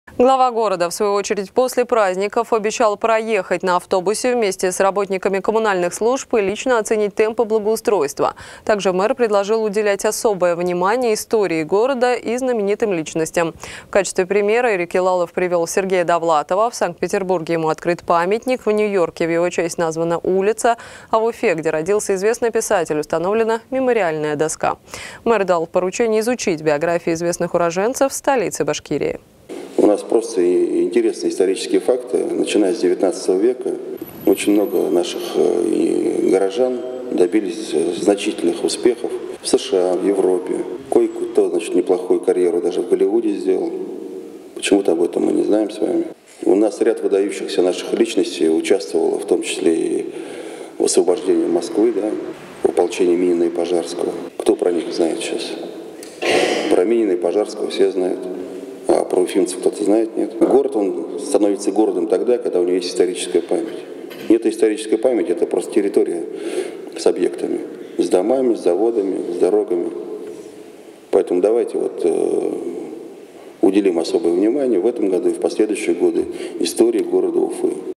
UTV. Ирек Ялалов призвал изучить биографии известных уроженцев Уфы
Ирек Ялалов  предложил уделять особое внимание истории города и знаменитым личностям. В качестве примера Ирек Ялалов привел Сергея Довлатова. В Санкт-Петербурге ему открыт памятник, в Нью-Йорке в его честь названа улицу. А в Уфе, где родился известный писатель, установлена мемориальная доска. Мэр дал поручение изучить биографии известных уроженцев столицы Башкирии.